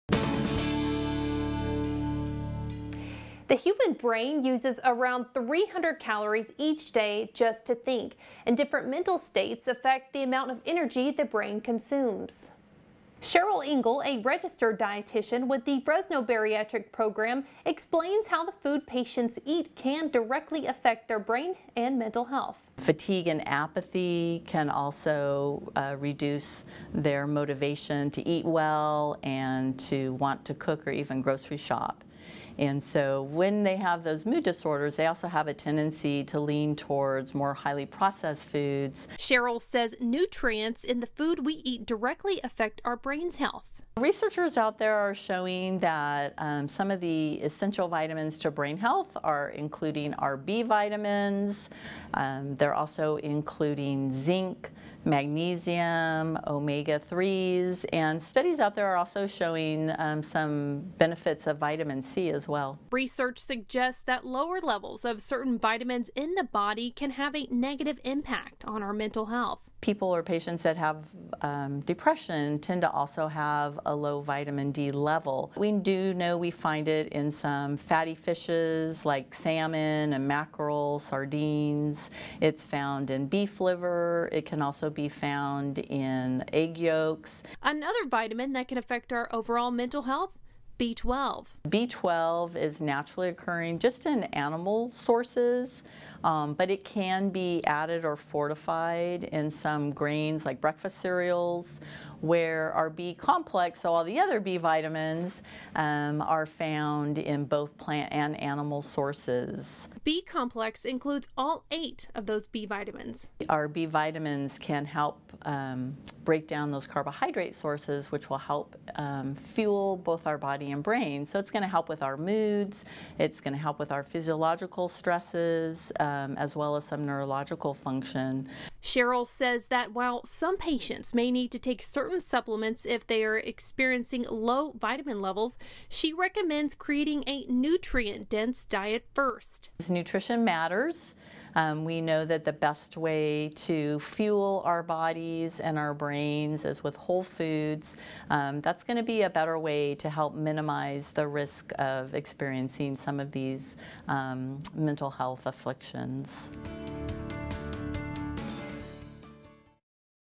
Vitamin Levels and Mental Health
Cheryl Engle, registered dietitian with Community Medical Centers, explains how the vitamins and minerals in the foods we consume can have an effect on our mental health.